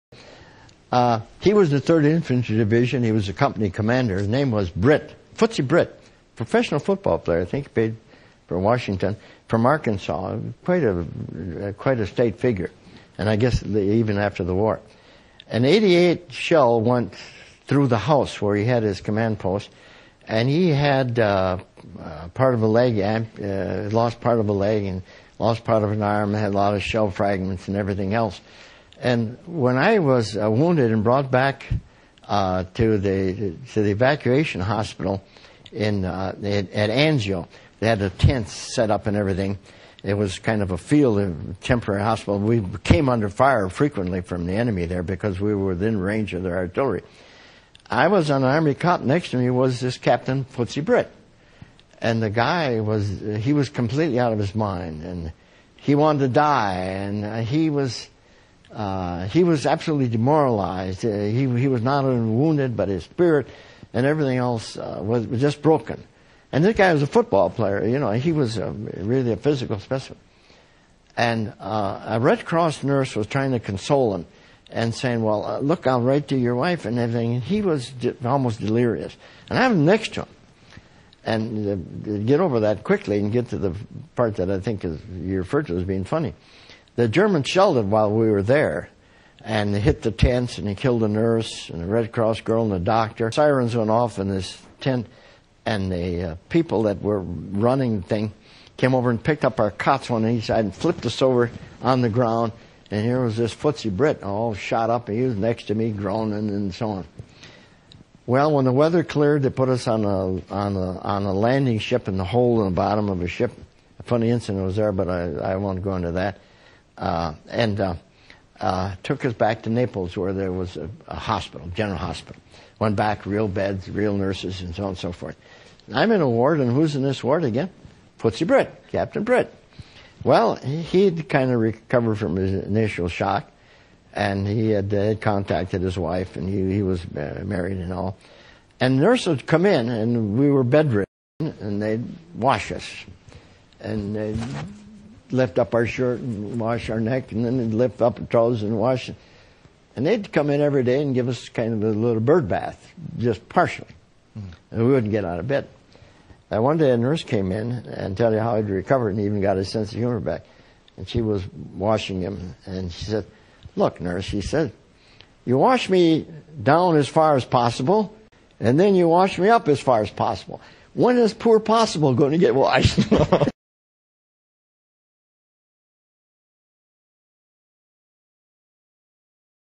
Capt. Maurice "Footsie" Britt - 3rd Infantry Division WW2
North Africa, Sicily, Southern Italy, Anzio.
Bronze Star, Silver Star, DSC, Medal of Honor